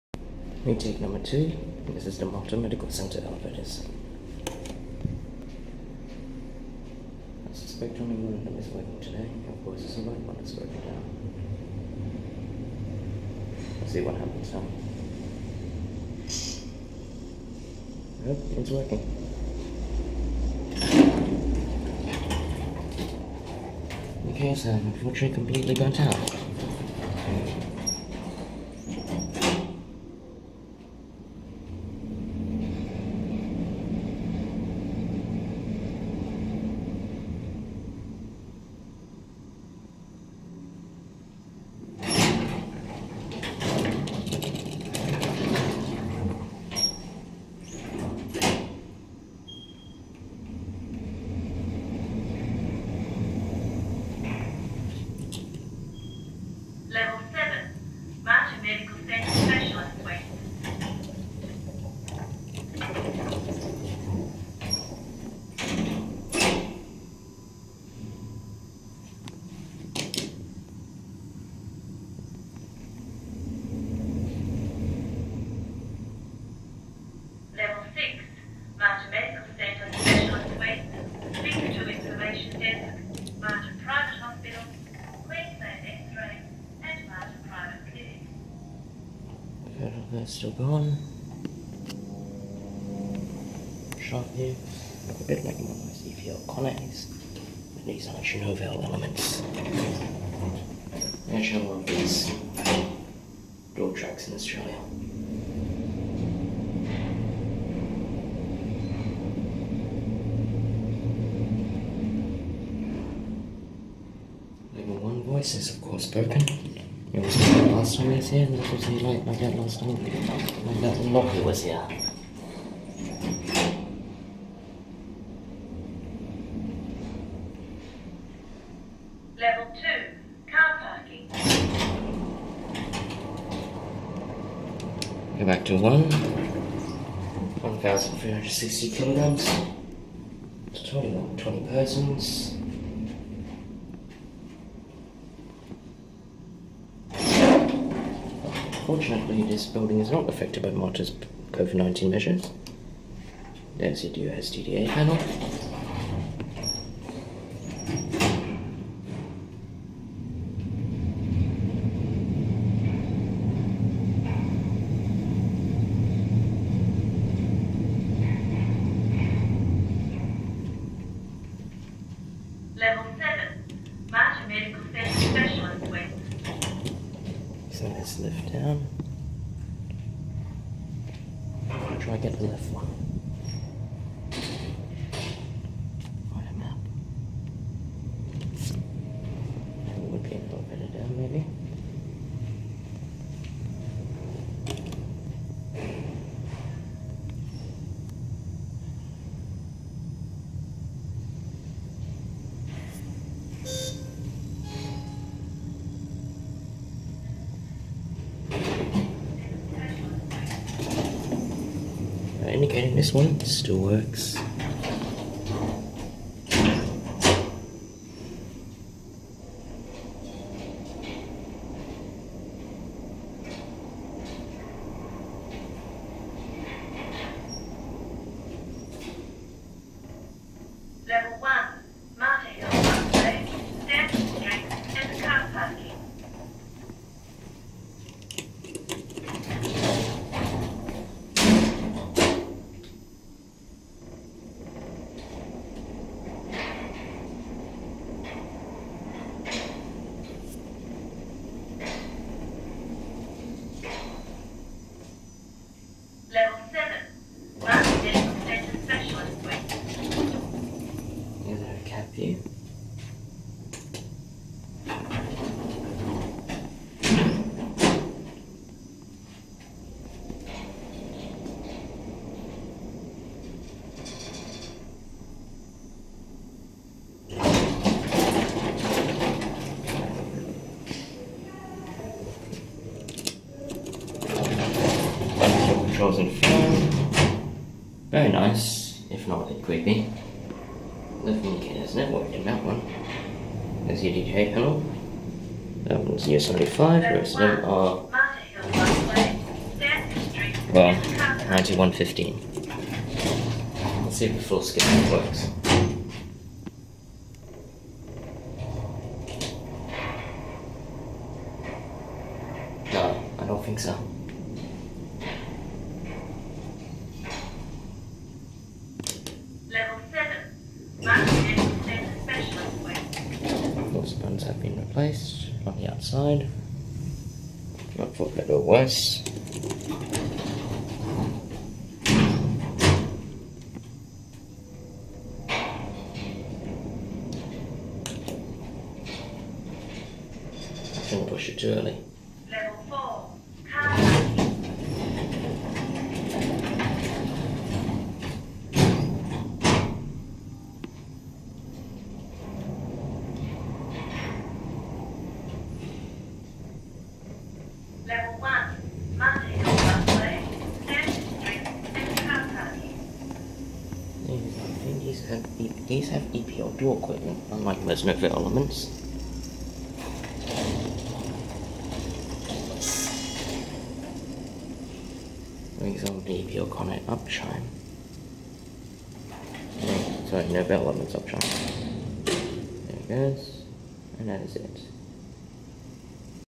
Useless indicators: [R2] 2 Kone Novel Elements Traction elevators at Mater Medical Centre, Sth Bris
As usual, the indicators outside are actually full Novel Elements indicators, and yet they only function as lanterns. They even still participate in the classic Novel Elements ping pong when they're bored. As for the inside indicators, the right elevator's left indicator is burnt out completely, but interestingly enough, as I discovered later on, the right one still works, but is very dim. The left one's indicator does still work too. The voice is also degrading further, no longer announcing Level 6 in the right car. It's quite sad because I actually like this one. I wonder how many Novel Elements got ELD indicators rather than the usual LED dot matrix the rest of the world got. This one, as usual, has had a lighter mod with a new Dewhurst US91-15 panel and call buttons, with presumably the voice being added in the mod (as she mentions the Mater Hill busway, which didn't exist in 1993); guess which one the public use more.

Brand: Kone
Type: Traction
Model: Novel Elements
Levelling description: Direct approach to floor
Capacity: 1,360 kg (20 persons, approx. 2,998 lb)
Fixtures: Kone Novel Elements (COP), Dewhurst US91-15 (LOPs), Dewhurst US91-15/US95 (DDA panel), EPL Synergy 2000 (inside indicators)
Year installed: 1993
Year modernised: 2000s
Floors served: 7 (1 - Mater Hill busway and exit, 2 - carpark, 3 - active rehab, 4 - carpark, 5 - upper entrance, 6 - link to Private Hospital and suites, 7 - suites)
Death-Trap rating: 2/10 (untrustworthy logic)

Camera used: Nikon Coolpix A900
Resolution: 1920x1080p30
Video 2 of 4 from October 24, 2020 DFO Jindalee, South Bank and Mater Hill.